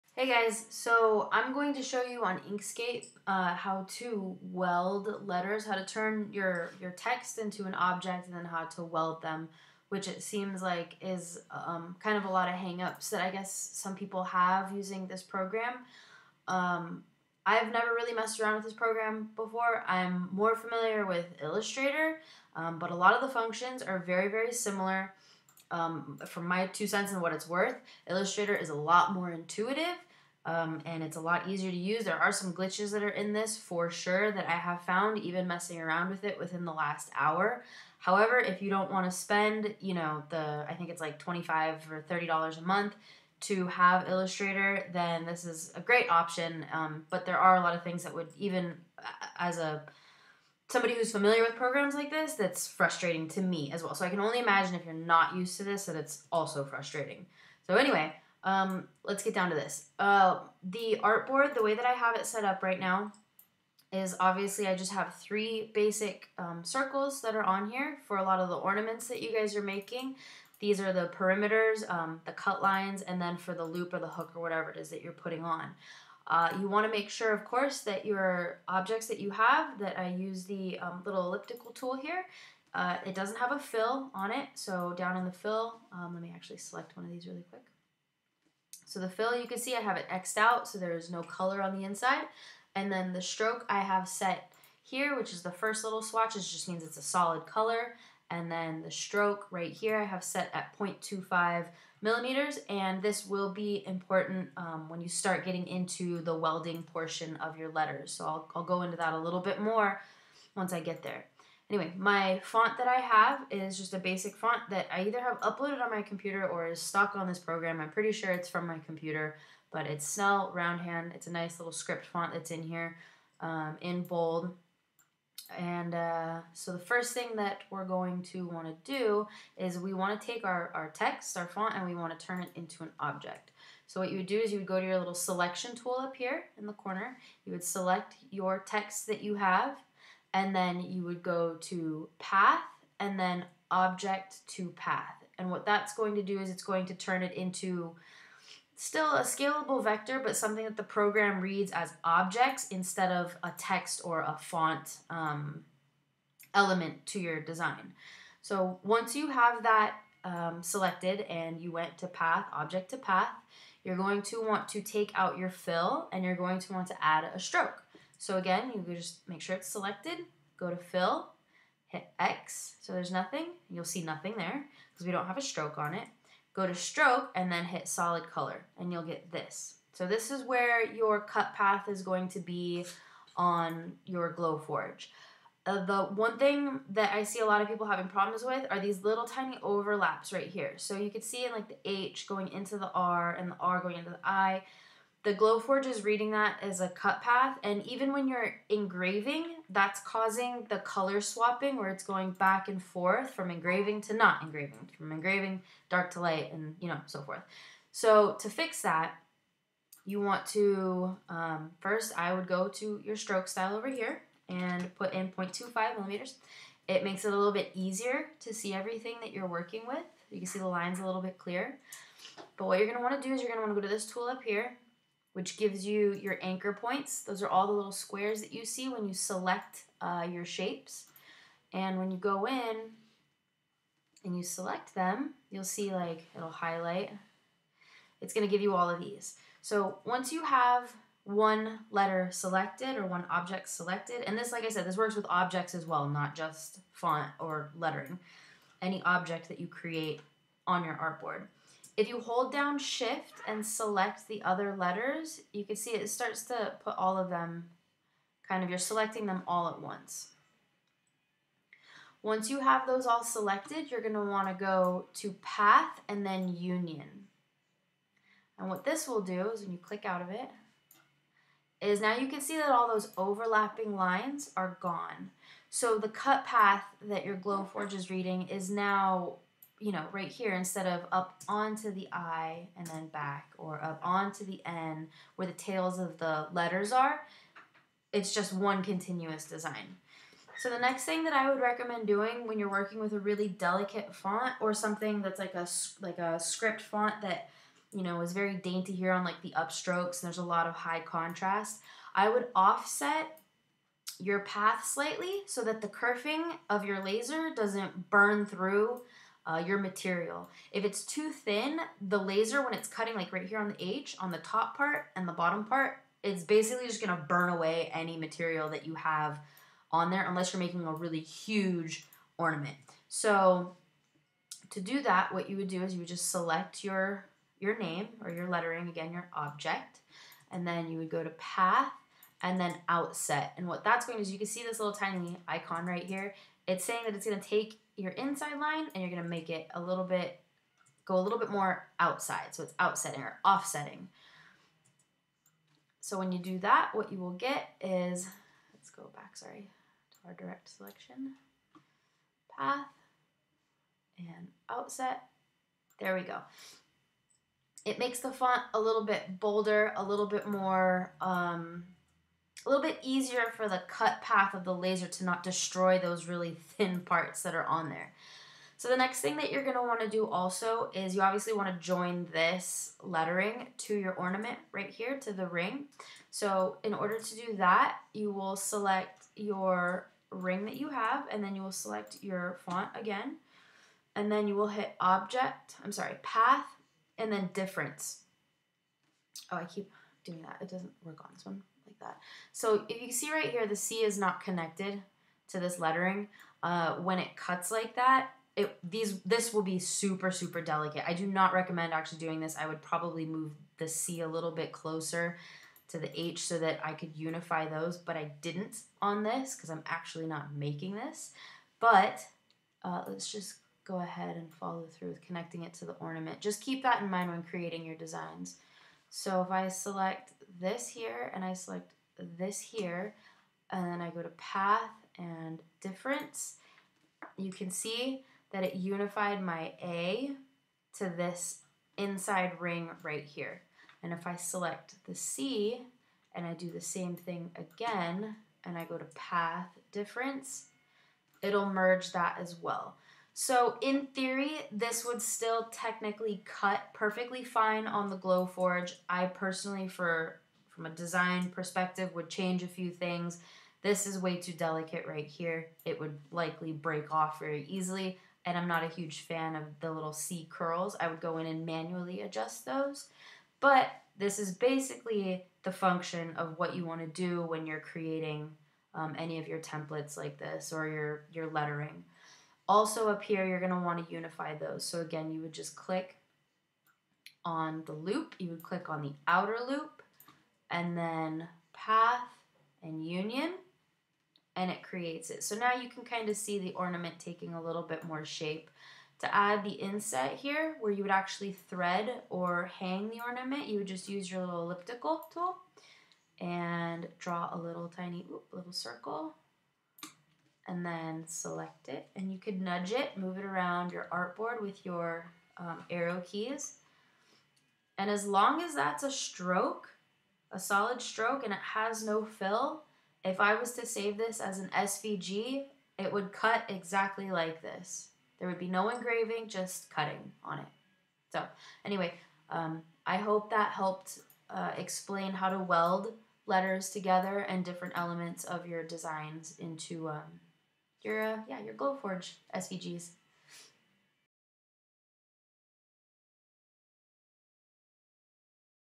How to Weld/Unite and Create a Path Offset In Inkscape
Quick tutorial on "welding" letters in inkscape and offsetting the path.

Please note that this was just an example to show the TOOLS used, this design has a few flaws (ie, the tittle..yes thats a word...over the "i" would not work on a laser cutter, as well as the C not being connected to the -hristina). The C would technically be welded/joined to the inner perimeter, but the rest of the name would be subject to breaking since its only connected at the end "a".

Also, I HIGHLY suggest looking into Illustrators capabilities. While Inkscape is great for a free tool, the interface and ease of use with OUT glitches is worth the $ alone.